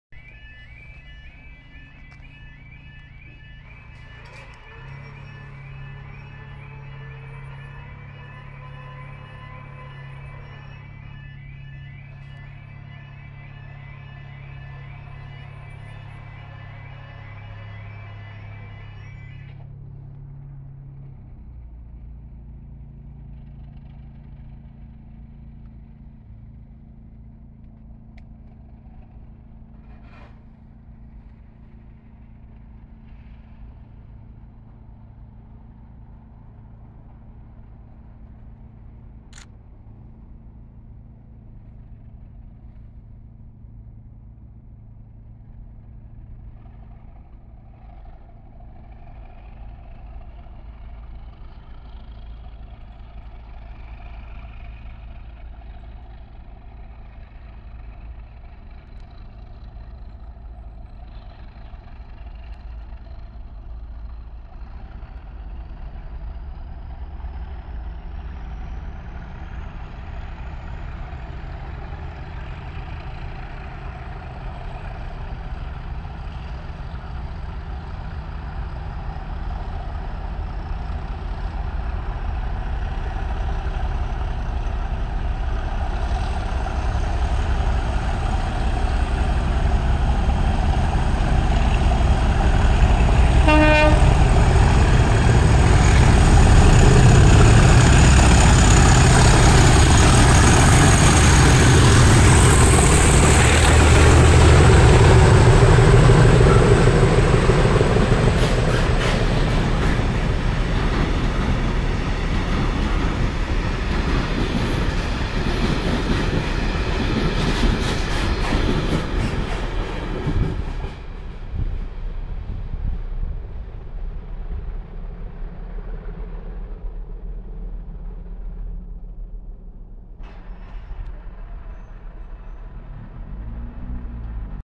97302 + 37254 standing start from Wrenbury 6C73 Crewe Basford Hall to Welshpool 17/04/23
97302 "Ffestiniog & Welsh Highland Railways/Rheilffyrdd Ffestiniog ac Eryri" + 37254 "Cardiff Canton" are seen here working 6C73 Crewe Basford Hall to Welshpool from a standing start at Wrenbury due to the late running 2J74 Crewe to Shrewsbury in front of it.